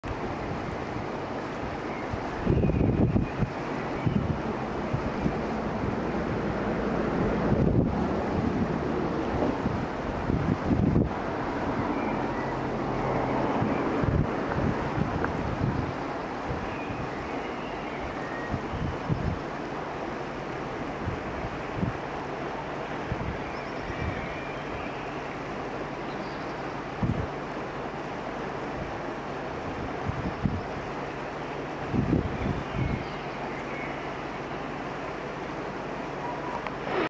Heron in Clausen
April 2020 - post-Covid I
While we waited for the lovely Restaurant Dal Notaro pizza, the heron ate a fish and a lizzard.